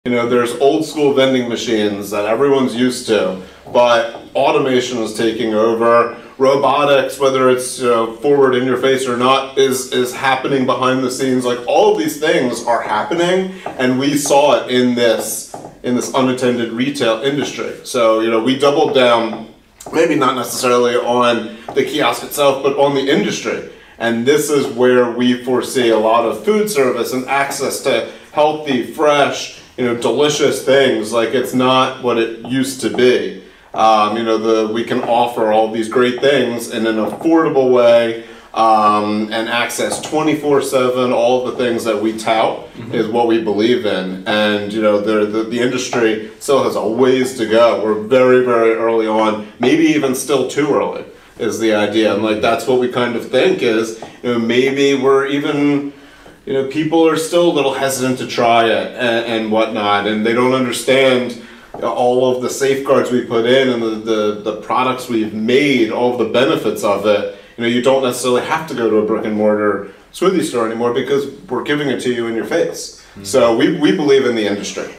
Unattended Retail: The Future of Food & Drink?
Automation and robotics are transforming the vending machine industry, offering 24/7 access to healthy, fresh, and affordable food. But are consumers ready to embrace this new reality?